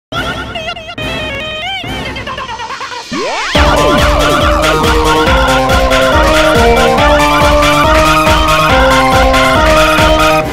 [Preview] Mickey Short Sparta Remix [Finish?]
Yes I know another preview...
This was a very old project, I just decided to fix a few things.
I don't know if I should finish this. EDIT: It's Done!